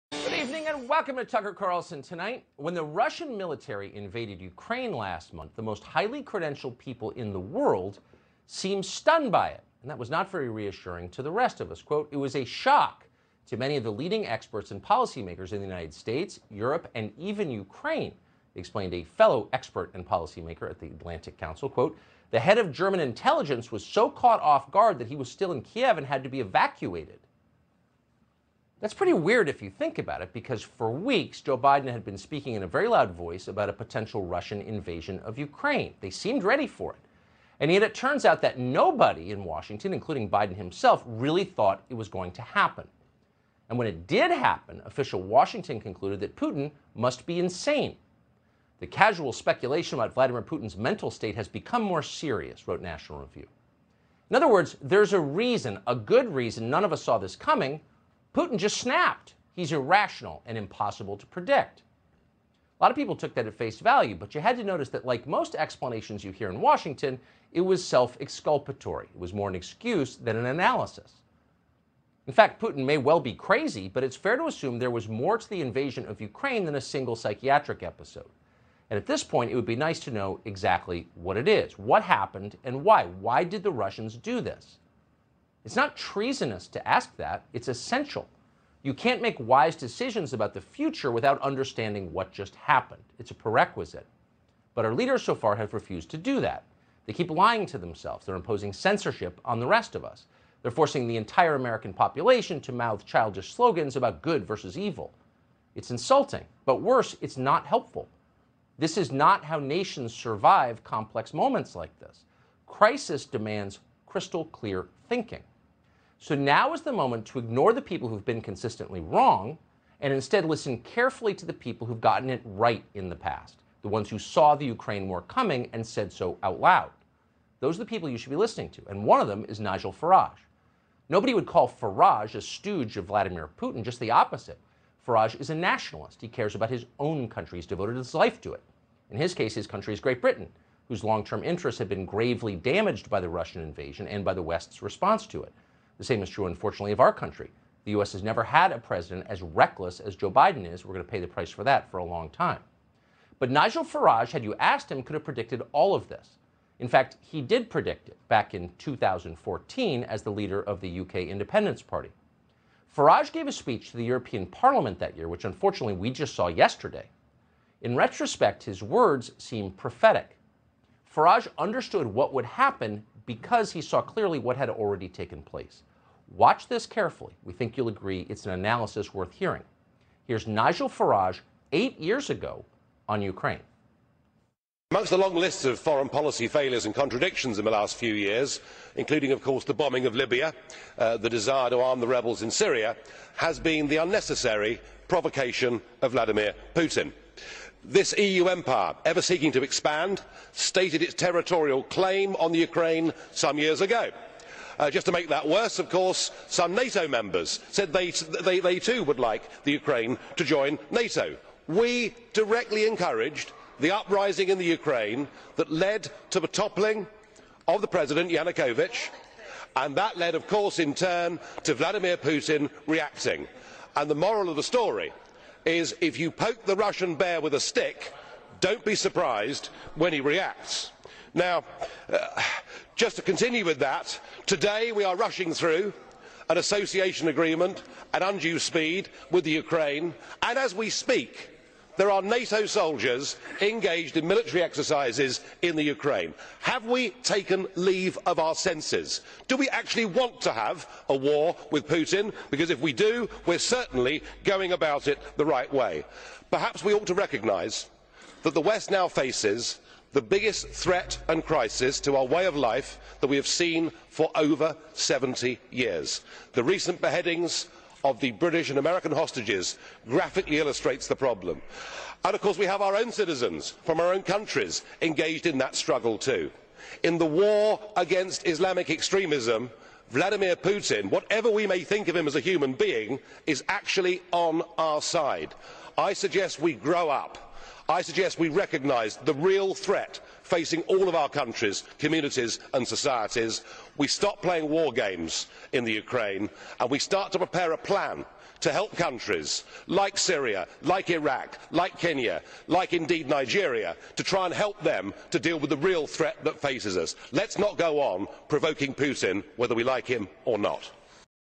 Tucker Carlson: Nigel Farage predicted the Current crisis in 2014 in the GB Parliament (ing - subt)
Tucker Carlson: Nigel Farage predijo la crisis actual de Ucrania en 2014 en el Parlamento de Gran Bretaña
ingles, subtitulado español